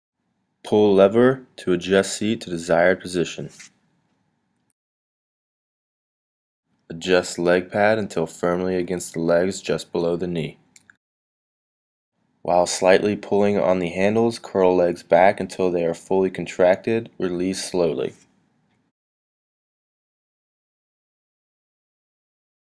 How to use the Hoist Leg Curl machine at the ASU Red WOLF Center
This is an instructional video on how to use the Hoist Leg Curl machine at the Arkansas State University Red WOLF Center. This video was filmed by the ASU Red WOLF staff.